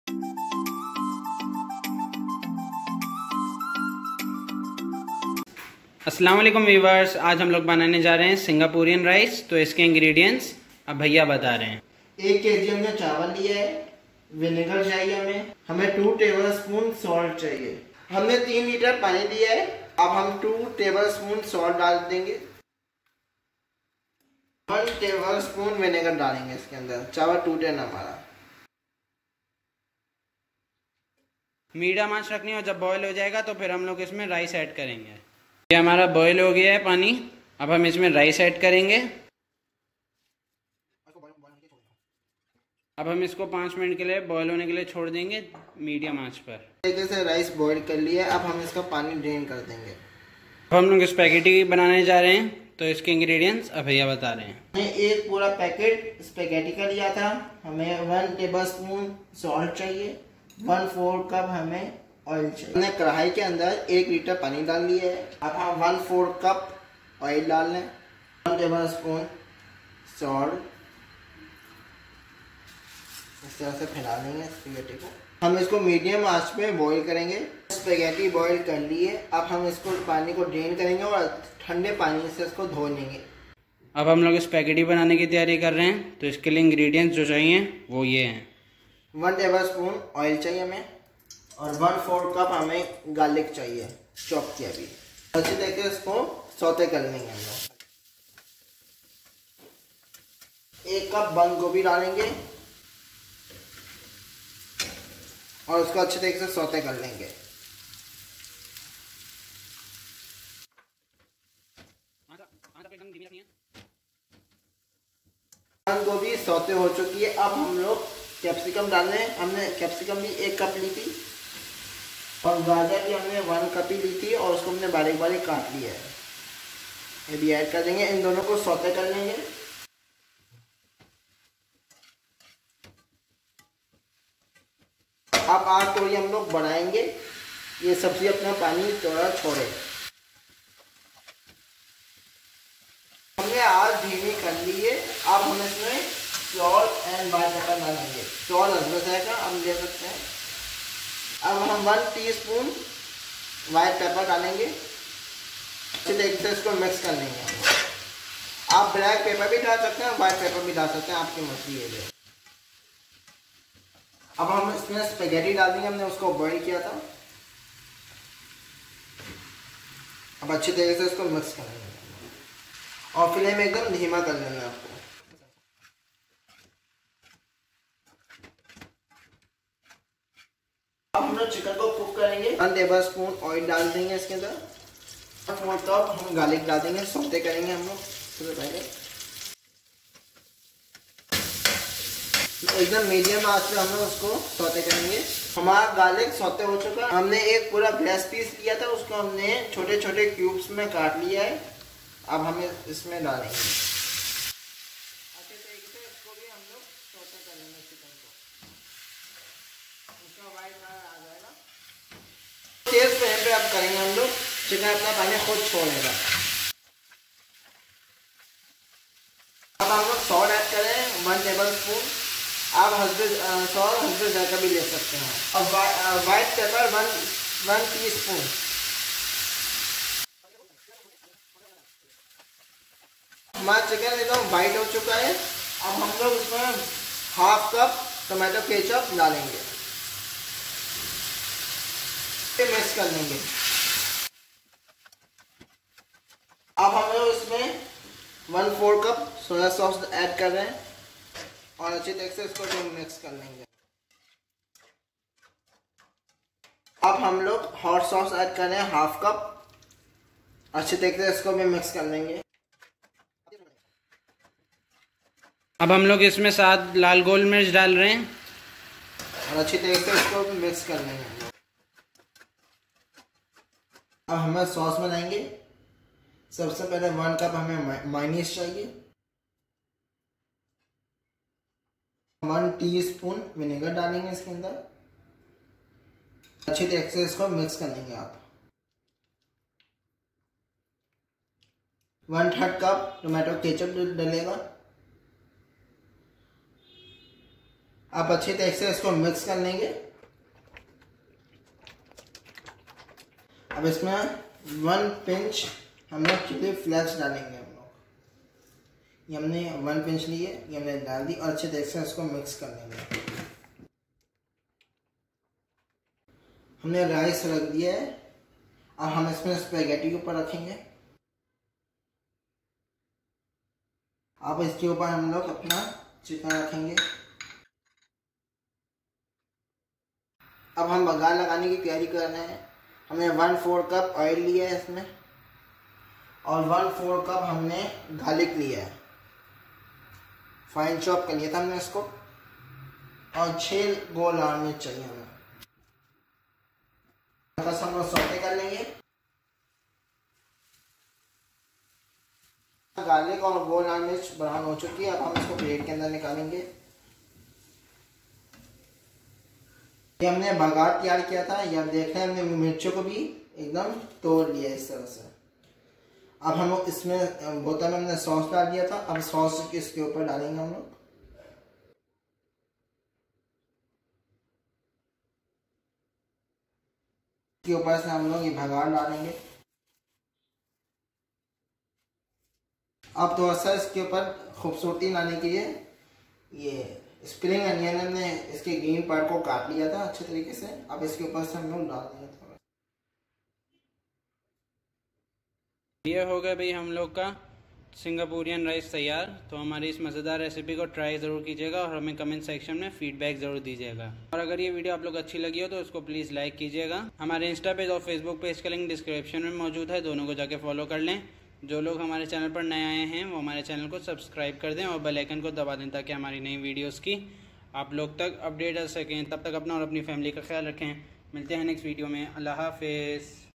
How To Make Singaporean Rice | Singaporean Rice In 8 Minutes | Chinese | Bihaar Cooks | Homemade
INGREDIENTS:-
1. TAKE 1 KG RICE.
2. TAKE 1 tbsp VINEGAR.
3. TAKE 2 tbsp SALT.
4. TAKE 3 LITRE WATER.
FOR SPAGHETTI:-
1. TAKE 1 PACKET OF SPAGHETTI.
2. TAKE 1 tbsp SALT.
3. TAKE ¼ CUP OIL.
4. TAKE 1 LITRE WATER.
5. ADD 1 tbsp OIL.
6. TAKE ¼ CUP CHOPPED GARLIC.
7. ADD 1 CUP CABBAGE.
8. ADD 1 CUP CAPSICUM.
9. ADD 1 CUP CARROTS.
10. ADD SALT AS YOU LIKE.
11. ADD 1 tsp WHITE/BLACK PEPPER.
12. ADD BOILED SPAGHETTI.
FOR CHICKEN:-
1. ADD 1 tbsp OIL.
2. ADD ¼ CUP GARLIC.
3. TAKE 1 BREAST PIECE OF CHICKEN AND CUT INTO CUBES.
4. ADD 1 tbsp SALT.
5. ADD 1 tsp WHITE PEPPER.
6. ADD 1 CUP TOMATO KETCHUP.
7. ADD ¼ CUP SOYA SAUCE.
8. ADD ½ CUP HOT SAUCE.
9. ADD 7 WHOLE RED CHILLIES.
FOR SAUCE:-
1. TAKE 1 CUP MAYONNAISE.
2. ADD 1 tsp VINEGAR.
3. ADD 1/3 CUP TOMATO KETCHUP.
4. ADD 1 PINCH CHILLI FLAKES.
FOR SIZZLE:-
1. ADD ¼ CUP OIL.
2. ADD ¼ CUP CHOPPED GARLIC.
3. ADD 6 WHOLE RED CHILLIES.

Musician: @iksonmusic